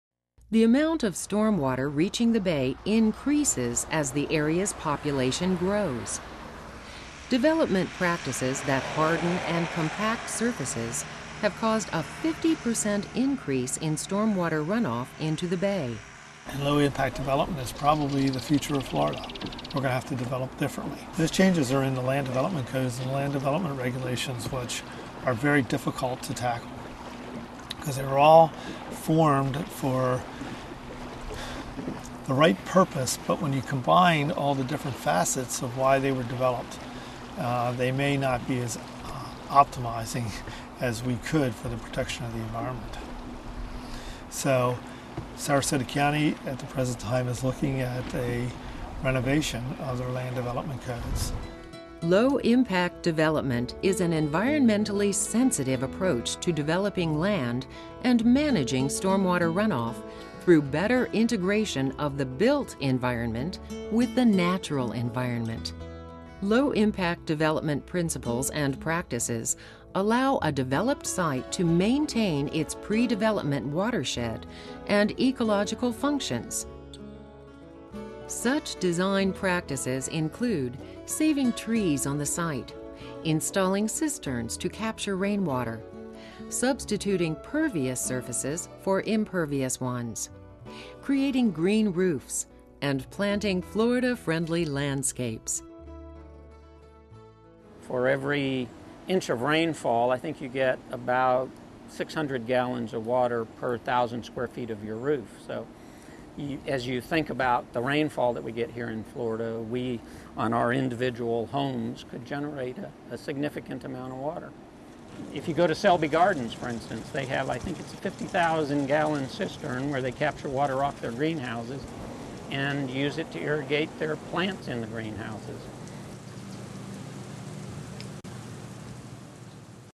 Low Impact Development
Low Impact Development (LID) is an environmentally sensitive approach to developing land and managing storm water runoff through better integration of the built environment with the natural environment.